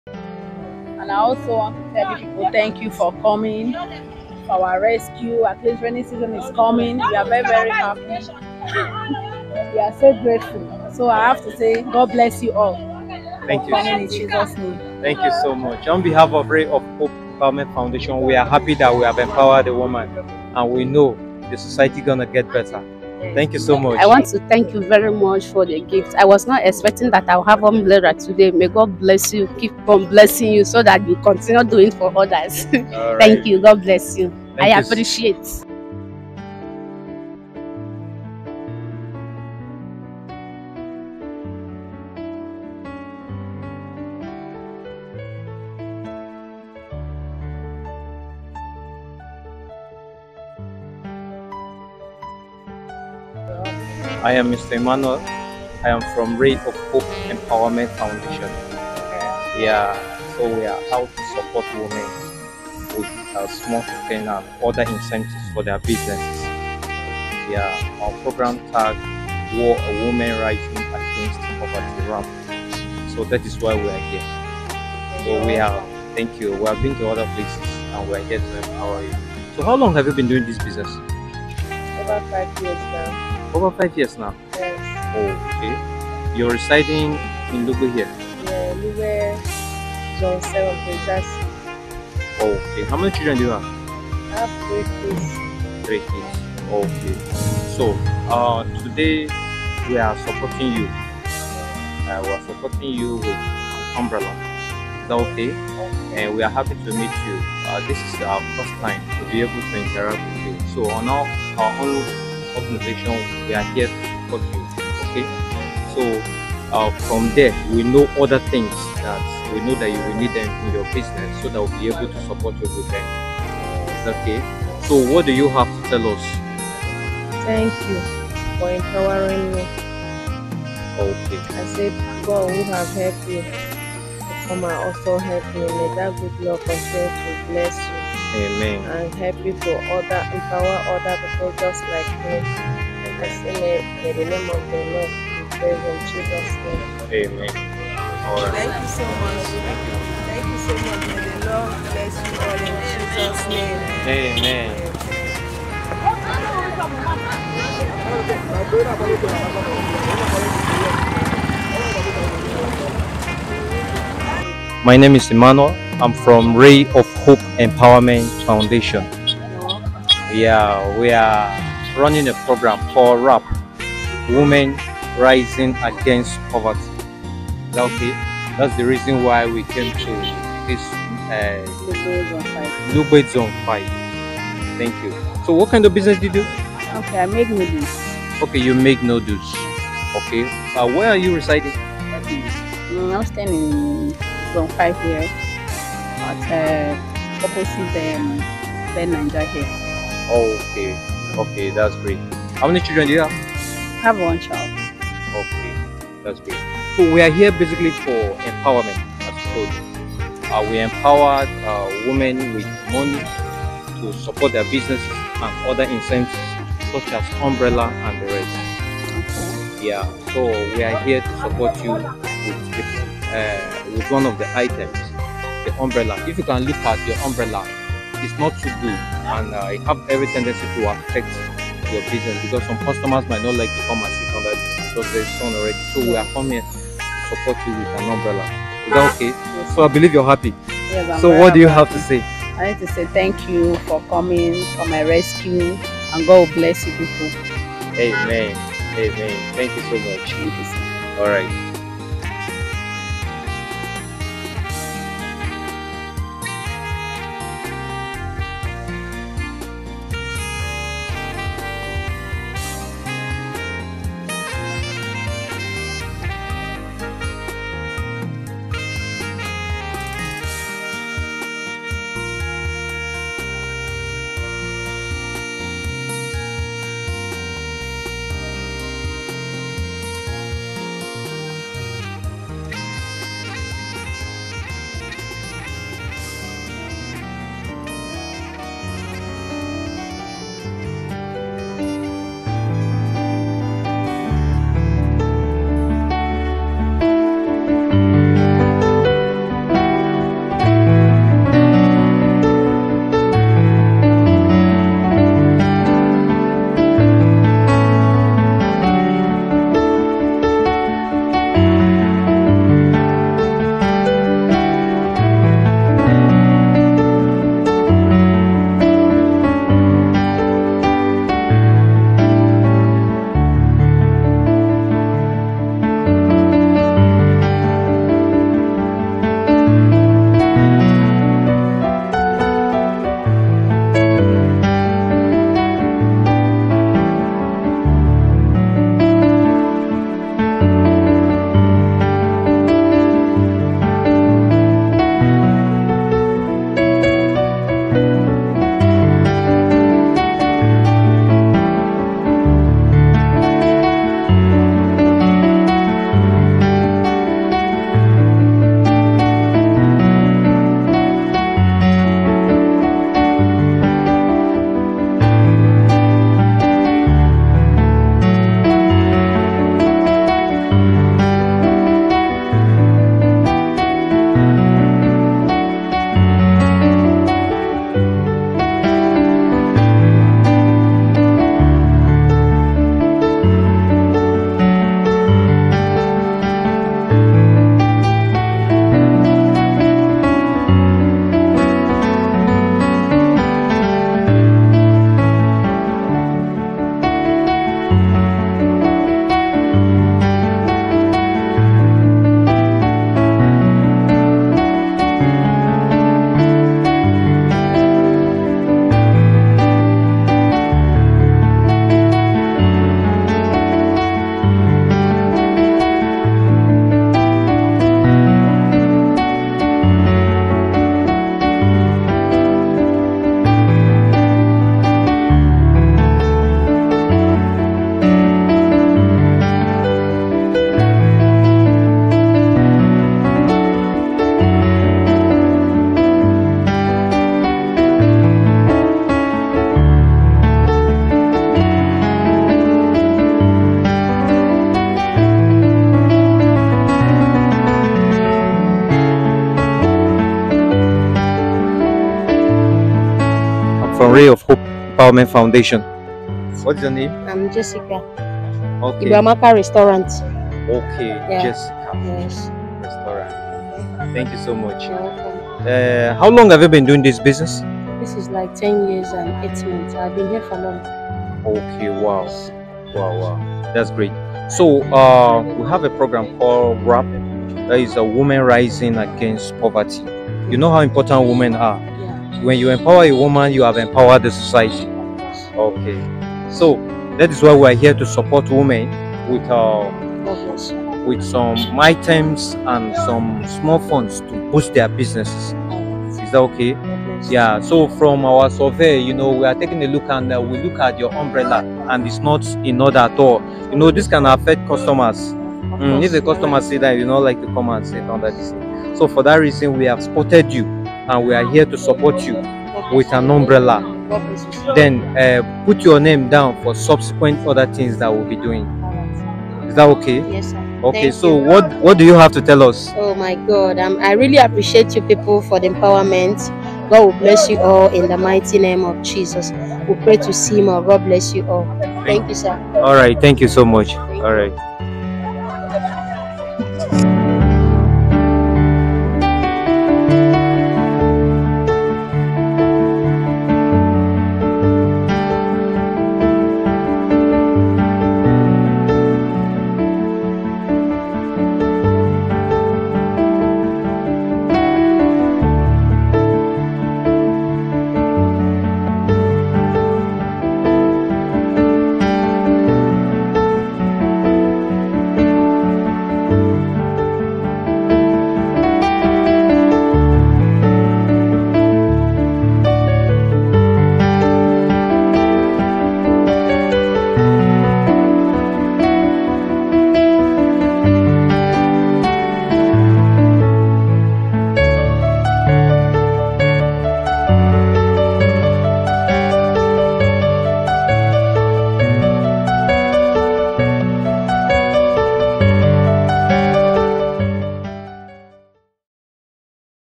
"Providing Shelter from the Storm: Donating Large-Sized Umbrellas to Underprivileged Market Women"
Ray Of Hope Empowerment Foundation, a non-profit organization committed to improving the lives of underprivileged women, recently donated large-sized umbrellas to help shield market women from harsh weather conditions.

The foundation, which is well-known for its dedication to empowering women and children through education, healthcare, and other forms of support, recognized that market women often work exposed to the sun and rain, which can lead to various health issues.

In response to this need, the foundation distributed large-sized umbrellas to market women in several underprivileged communities to help protect them from the sun and rain. The donation of these umbrellas not only provides physical protection from harsh weather conditions but also helps the women to continue their daily business activities uninterrupted.

This donation is just one of the many initiatives undertaken by the Ray Of Hope Empowerment Foundation to support underprivileged women and empower them to improve their lives and the lives of their families.